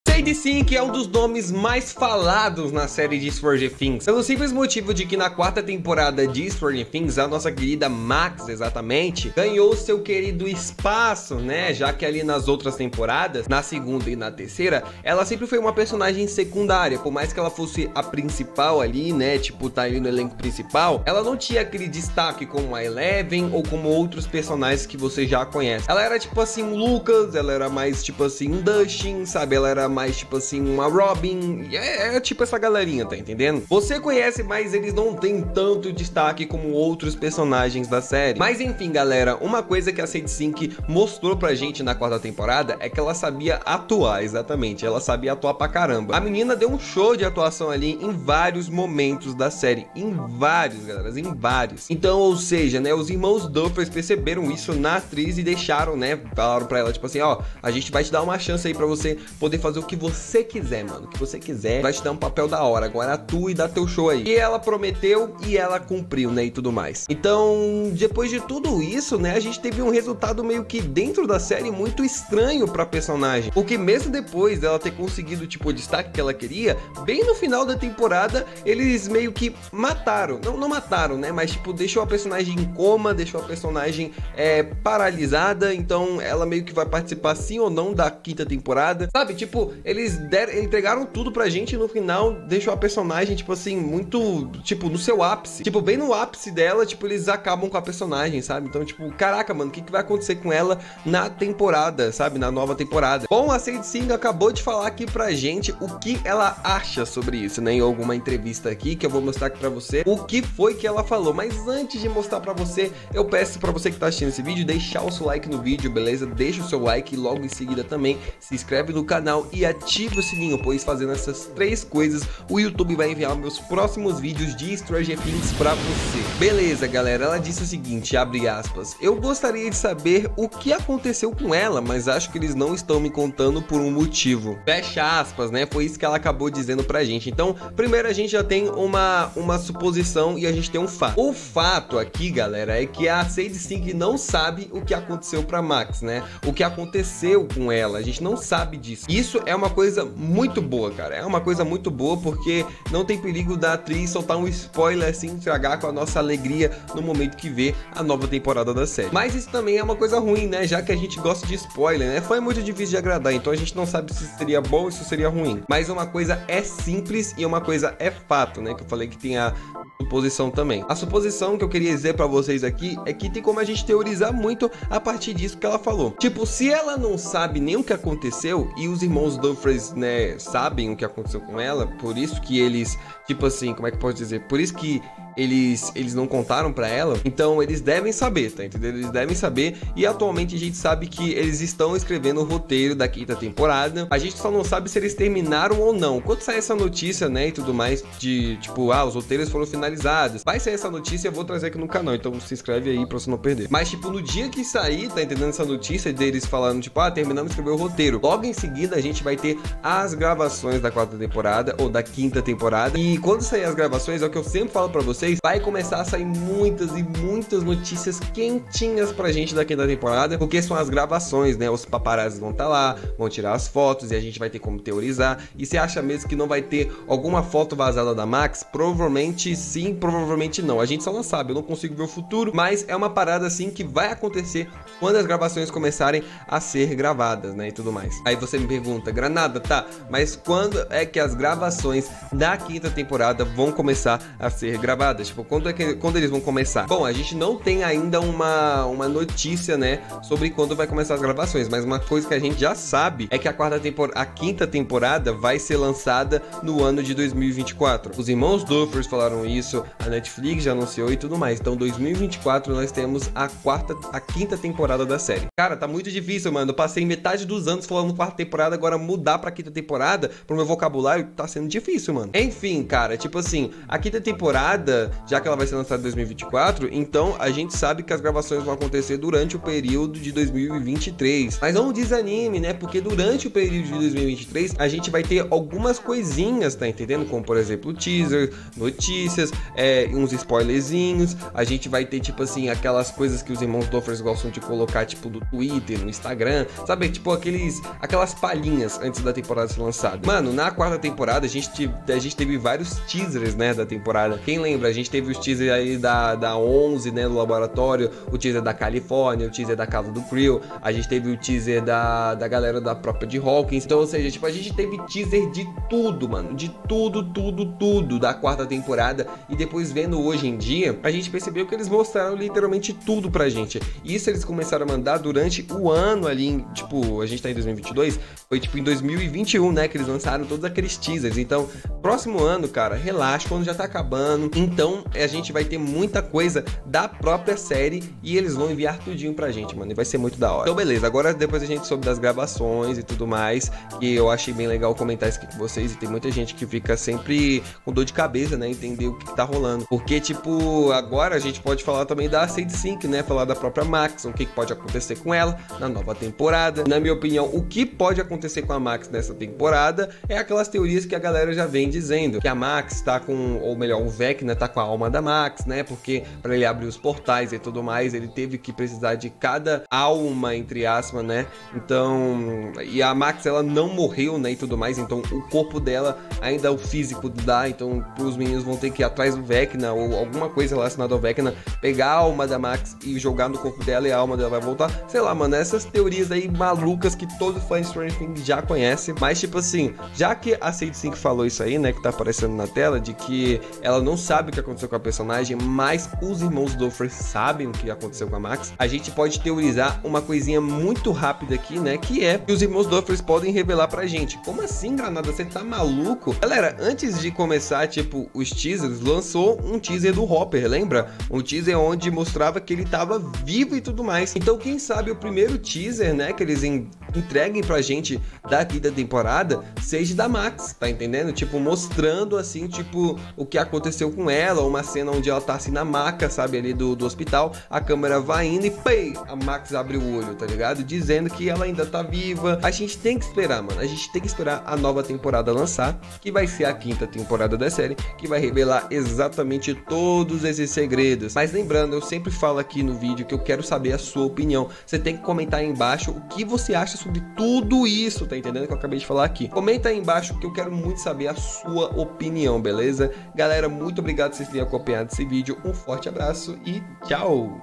Stranger Things 5 | BOMBA!! SADIE SINK ACABA COM O MISTERIO E REVELA O QUE SABE SOBRE A MAX
Sadie Sink revela que não sabe o que acontecerá com Max na 5ª e última temporada de StrangerThings